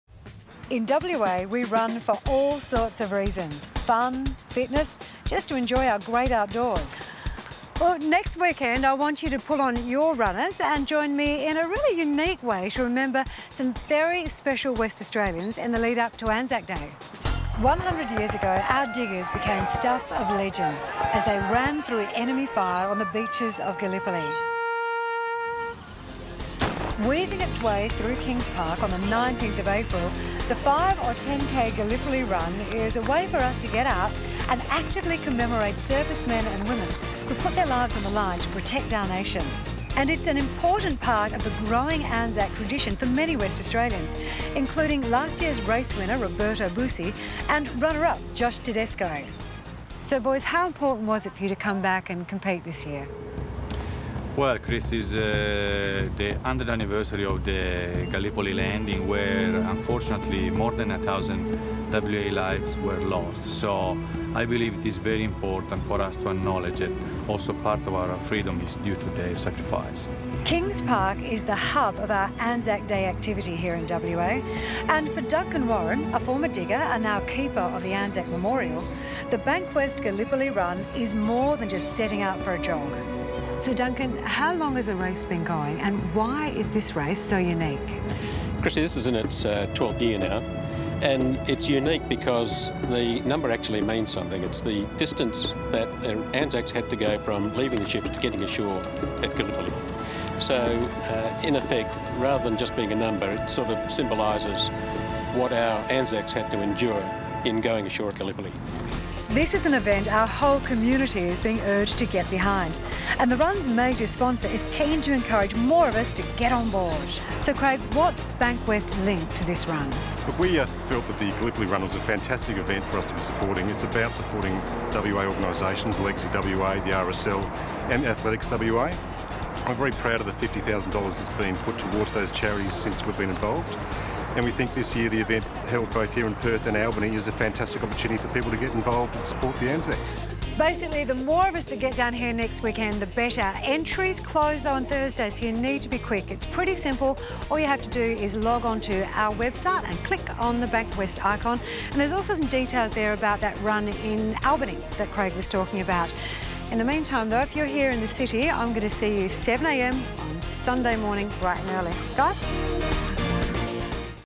Destination WA - Bankwest Gallipoli Run
Chrissy puts on her joggers to participate in an important event leading up to ANZAC day.